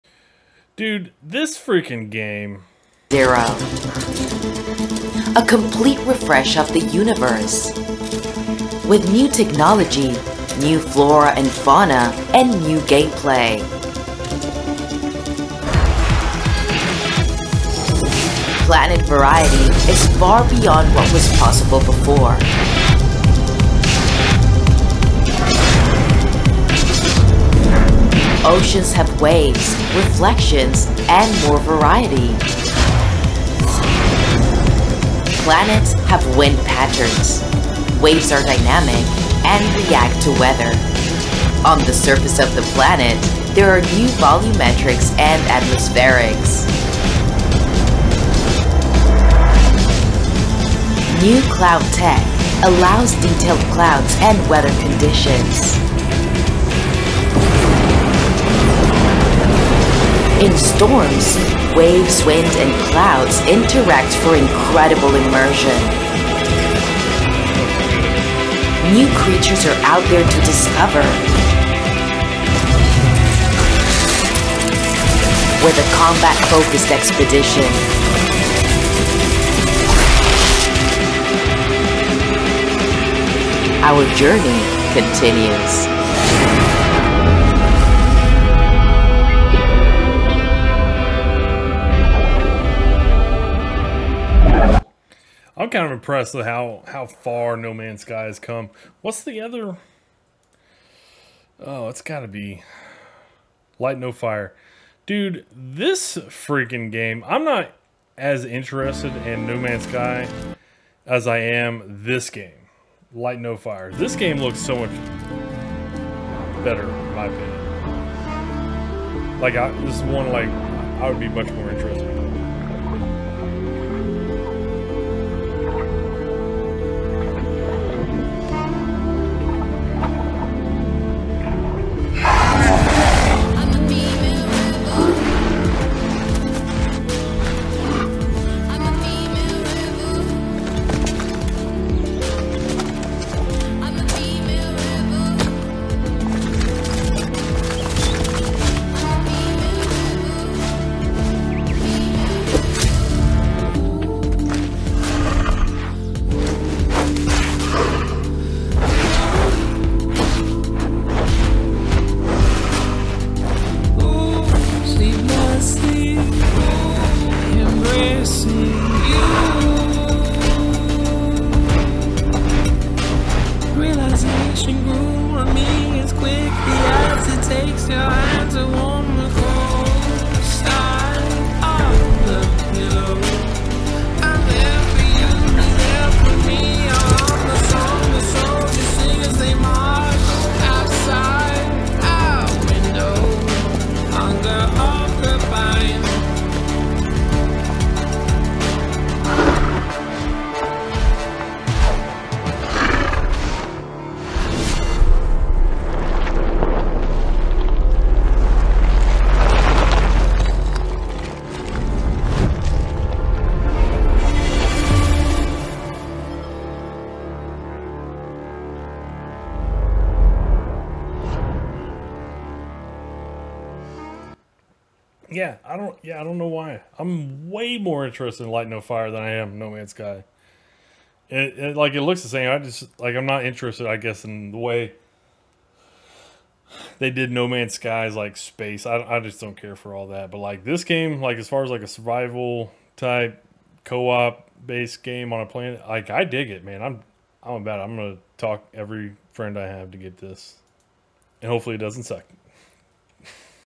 No Man's Sky has impressed but Light No Fire is what I'm looking forward to -- ShadowBadass
Both of these games really look impressive.

Twittter: @_shadowbadass